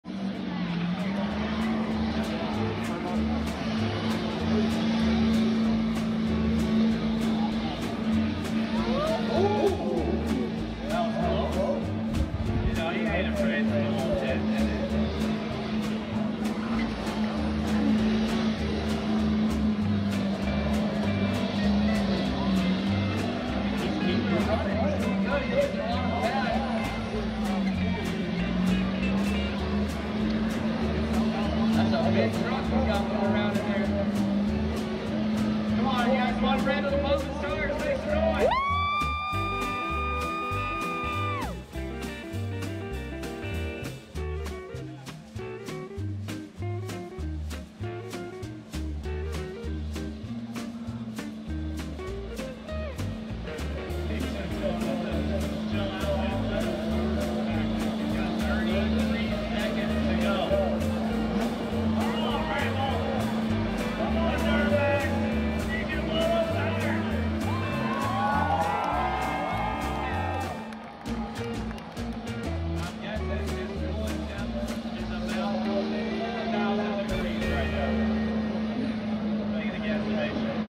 America
Drifting contest at Bristol Motor Speedway
Randy Young the Duramax